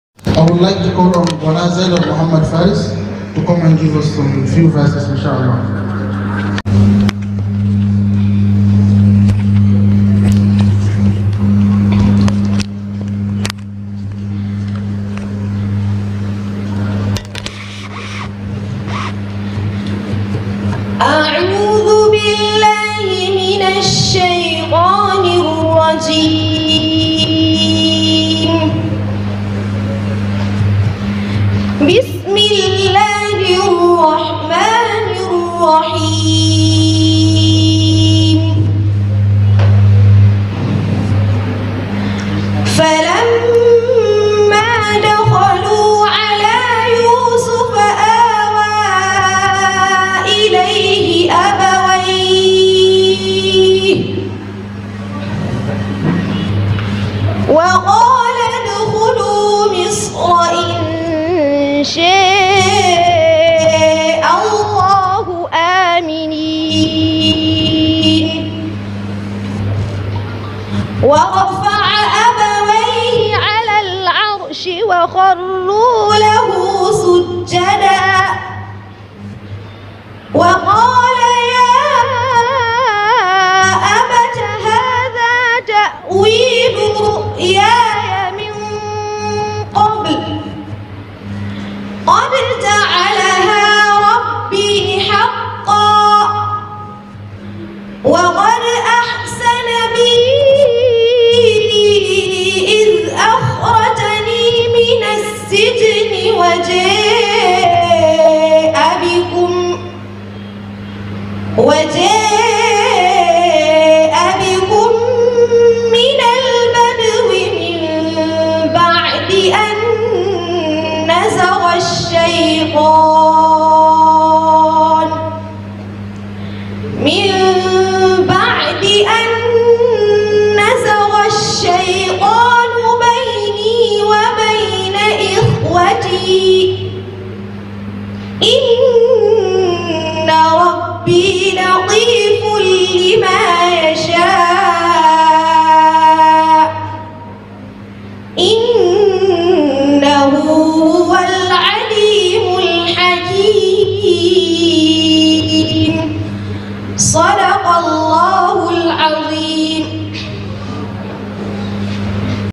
Karatun Al~Kur'ani Mai Girma - Gwana Zainab Muh'd Faris
1ST EDITION OF SHEIKH ALI AL-HUZAIFI QIRA'AT AWARD 2023

20th - 27th Rajab 1444AH.(11 - 18/02/2023) @Women Development Hall Center, Jimeta/Yola, Adamawa State